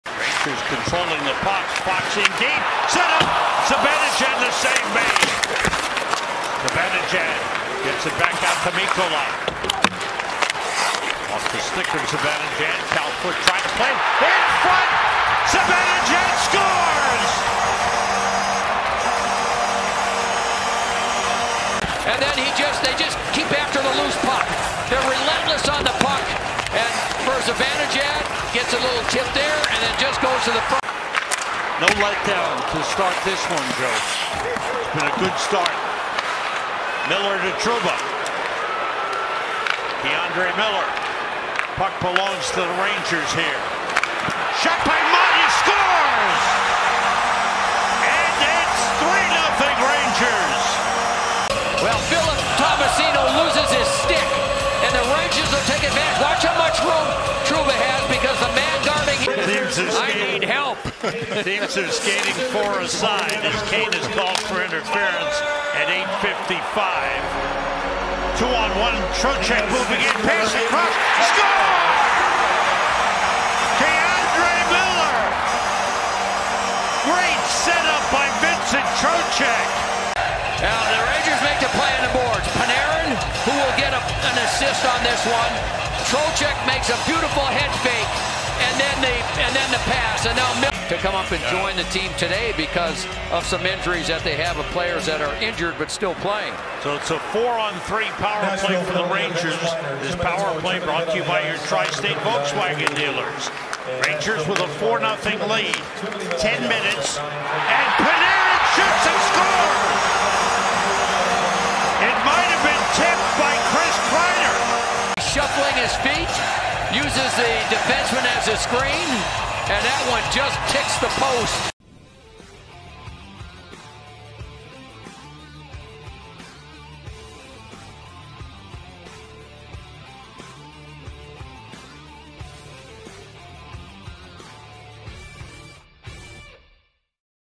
Rangers Score Four Goals In Under Three Minutes As Part Of Six-Goal First Period
Watch as the New York Rangers show off their offensive weapons with goals from Mika Zibanejad, Tyler Motte, K'Andre Miller, and Artemi Panarin in just two minutes and 21 seconds as part of their six-goal first period against the Nashville Predators.